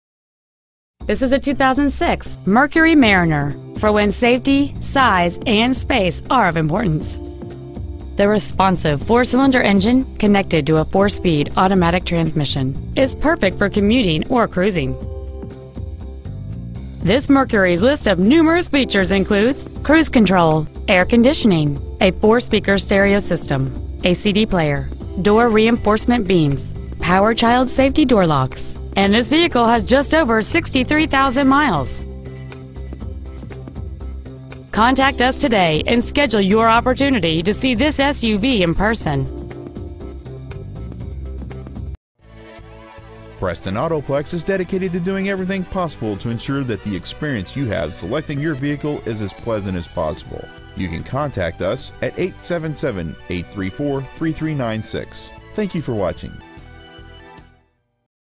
Preowned 2006 MERCURY MARINER Seaford DE
Year: 2006
 Make: MERCURY
 Model: MARINER
 Engine: 2.3L I4
 Trans.: AUTO 4SPD
 Exterior: SILVER METALLIC
 Miles: 63,533
 Interior: BLACK

 Used 2006 MERCURY MARINER Hurlock  MD